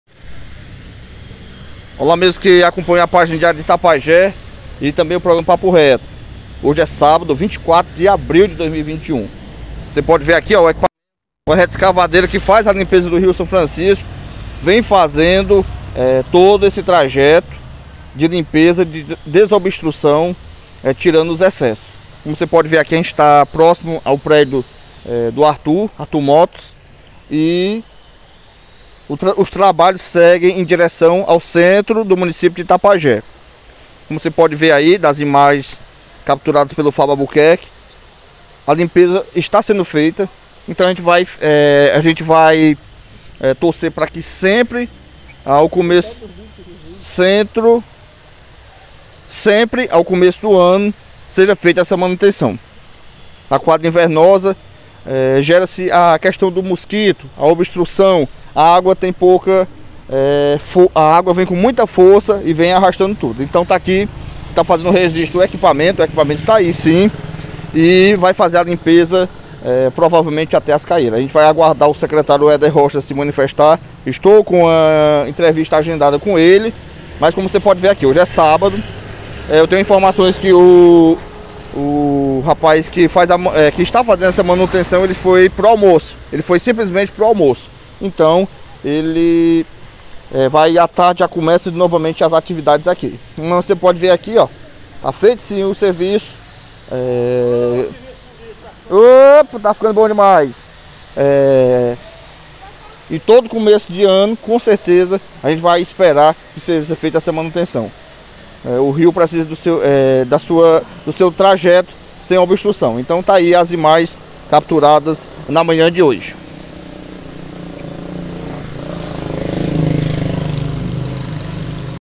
limpeza do Rio São Francisco chaga próximo ao centro de itapajé
Ainda manhã de hoje(24) acompanhando o serviço de limpeza do Rio São Francisco.
Acompanhe ⬇️